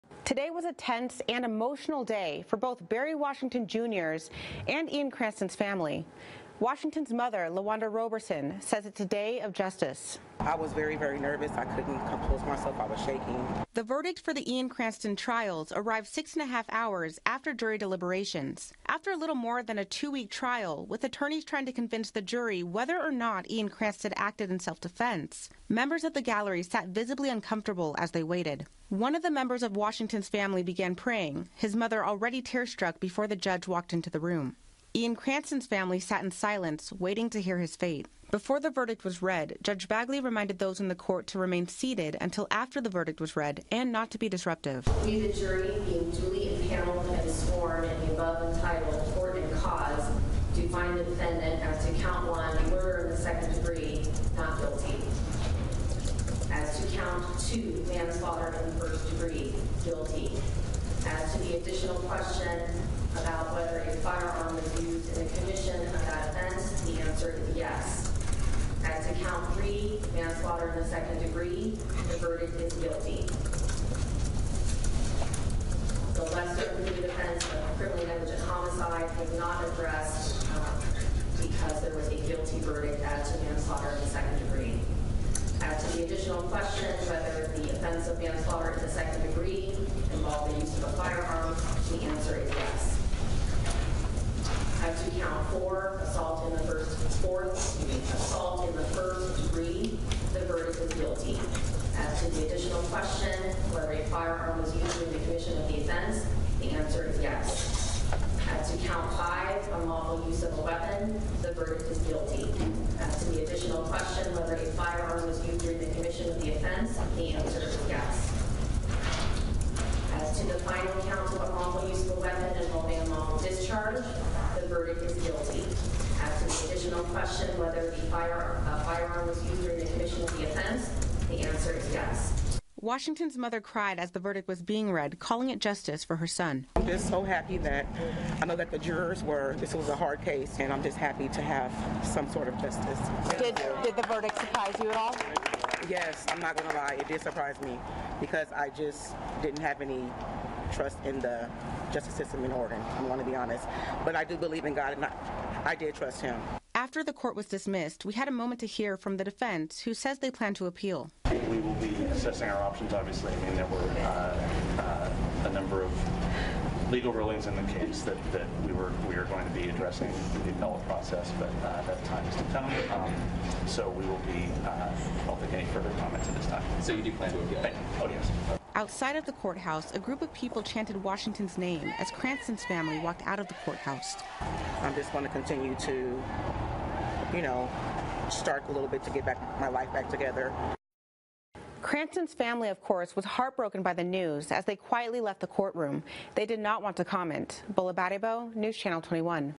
Jury returns verdict in Ian Cranston murder trial
A Deschutes County jury convicted Ian Cranston on a manslaughter charge for killing Barry Washington Jr. but not guilty on a second-degree murder charges